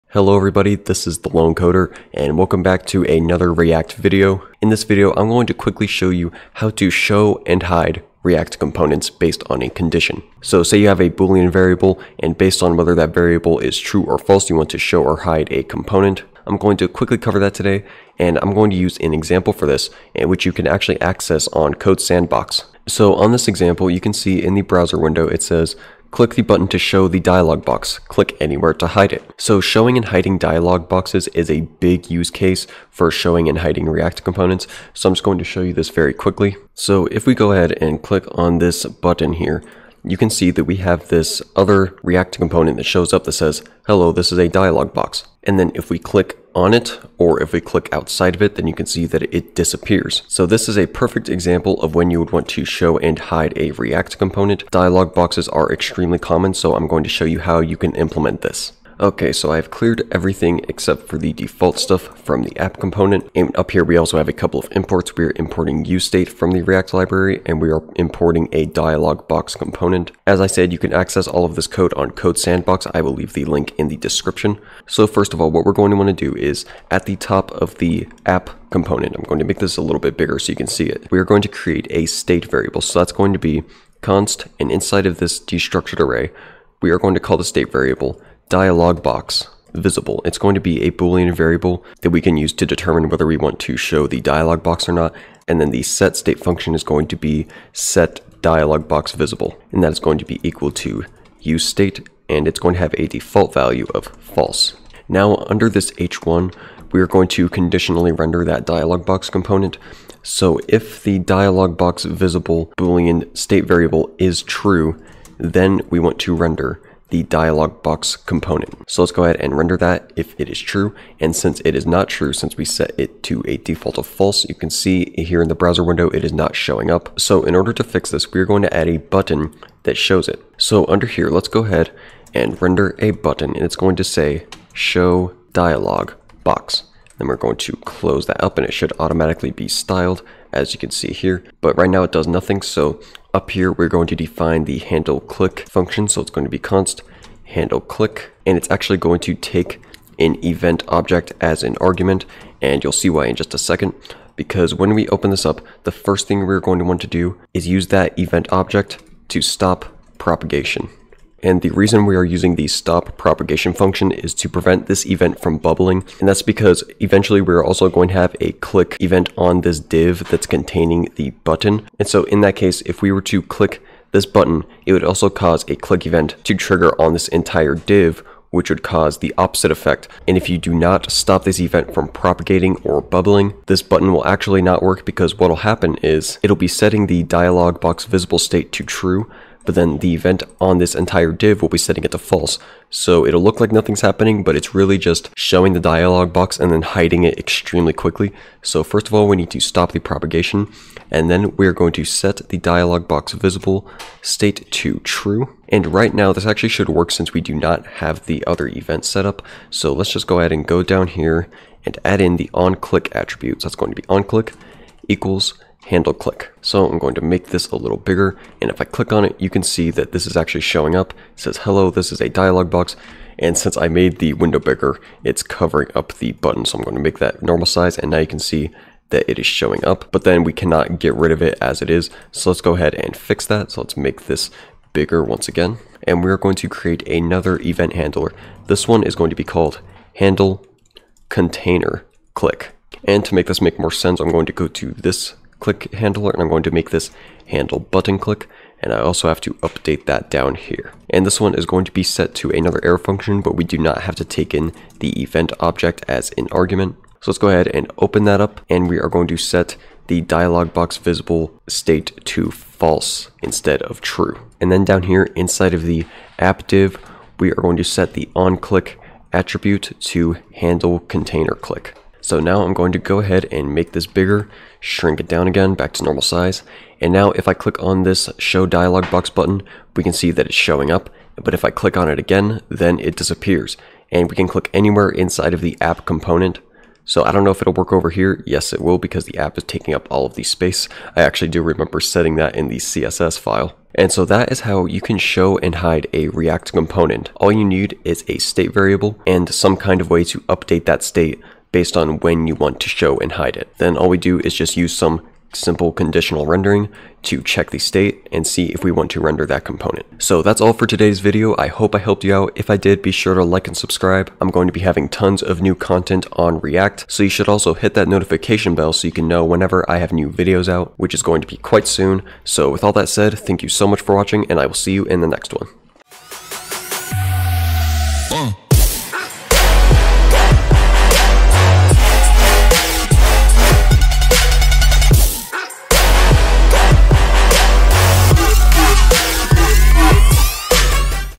How to SHOW and HIDE React Components | ReactJS Tutorial
Want to know how to easily show and hide React components based on a condition? Here's how.